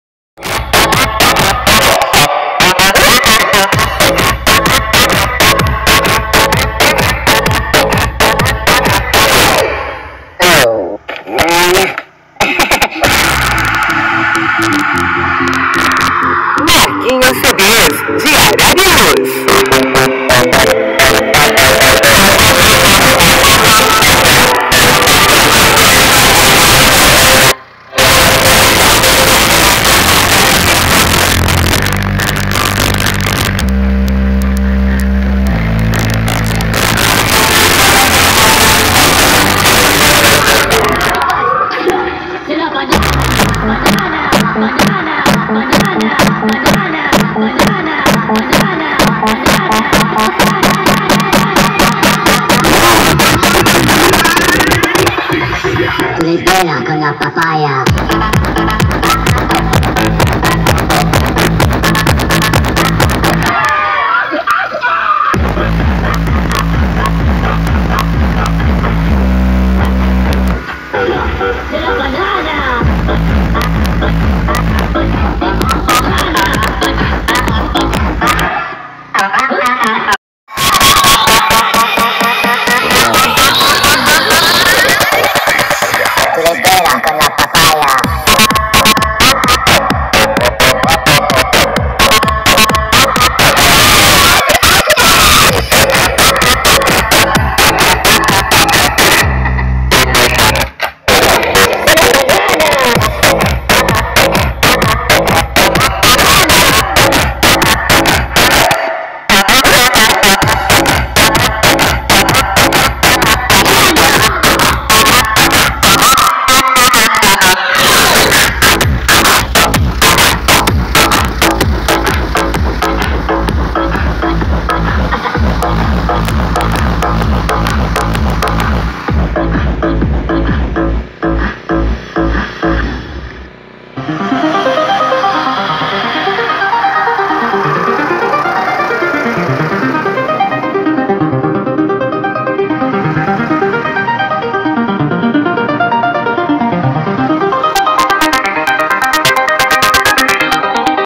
Som do Corsa Debochado - Projeto Transformers Quem Tem GRAVE É ELE Equipadora ((Qualitysom))
Projeto usando Eros 3k de 18 polegadas